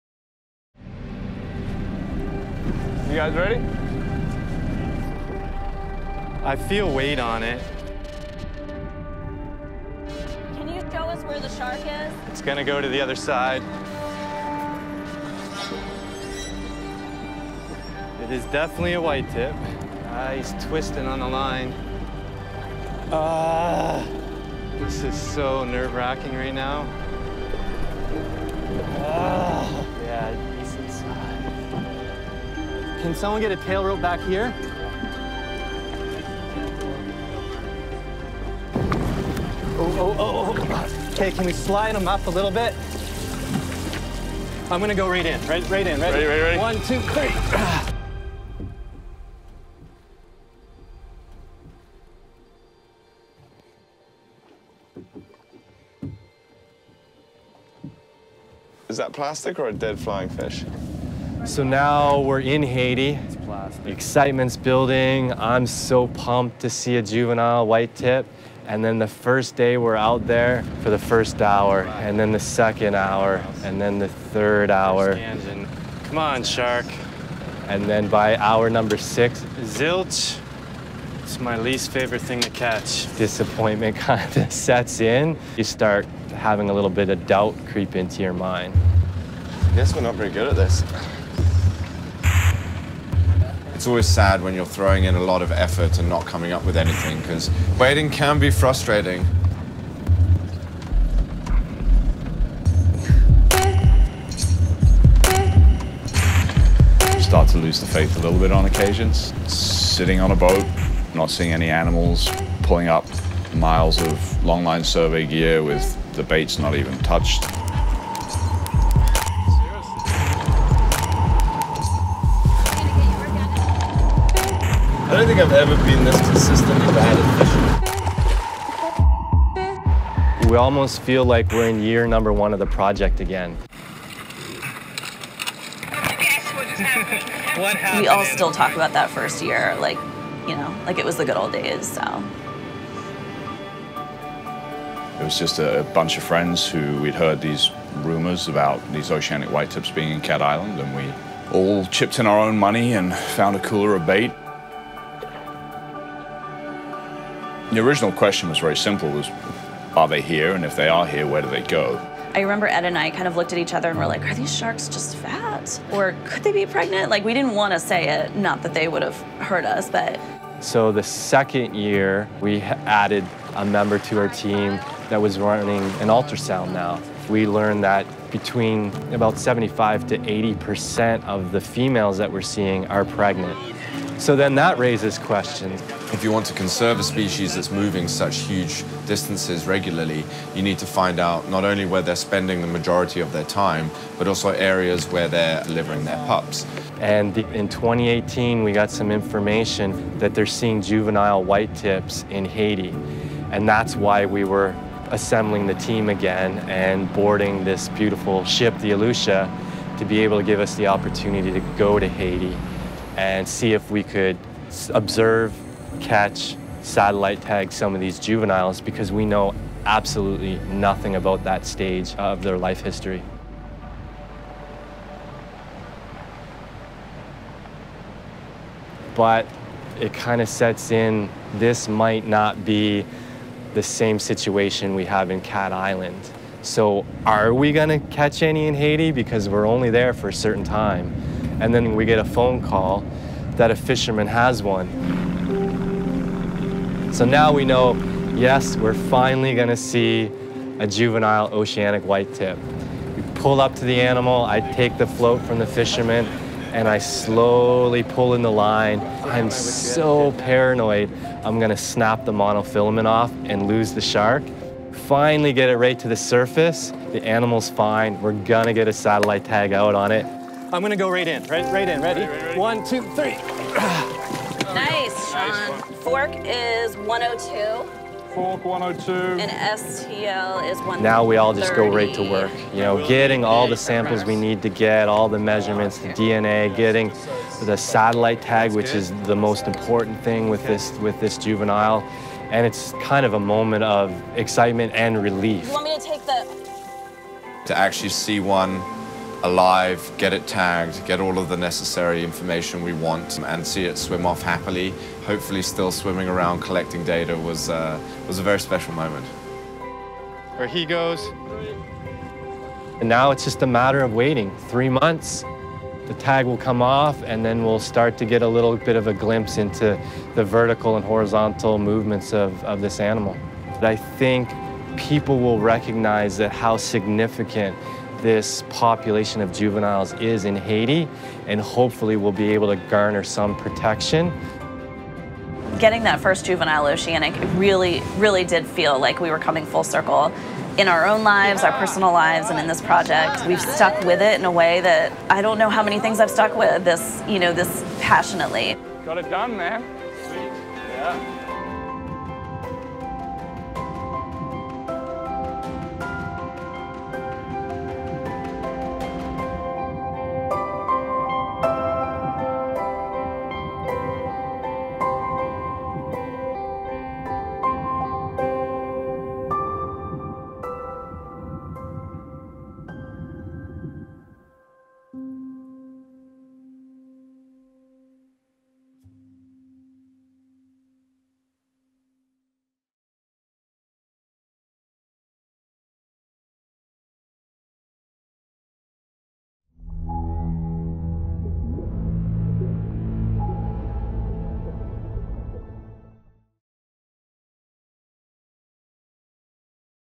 Finding Baby Oceanic Whitetip Sharks to Save Them From Extinction
Oceanic whitetip sharks were once so common, a 1969 textbook called them "perhaps the most abundant large animal, large being over 100 pounds, on the face of the earth." They're now suspected to be at a fraction of their former levels, as suggested by some scattered studies from different regions. This July, a group of scientists co-ordinated by the Cape Eleuthera Institute, OceanX, Haiti Ocean Project, and the Atlantic OWT Conservancy journeyed aboard OceanX's alucia to a suspected babyshark nursery just off the coast of Haiti.

Research by the Cape Eleuthera Institute, Atlantic Oceanic White Tip Collaborative, and the Haiti Ocean Project.

Director, Producer & Editor: Janssen Powers
Executive Producer: Mark Dalio
Director of Photography: Janssen Powers
Underwater DOP: Duncan Brake
Additional Camera: Andy Mann, Julien Jarry
Assistant Editors: Taj Howe, Patrick Hopkins
Post Supervisor: Gabe Tanenhaus
Social Producer: Erika Jarvis
Audio: Julien Jarry
Audio Mix: Max Sound Recording
Color Correction: Running Man Post
Production Associate: Patrick Hopkins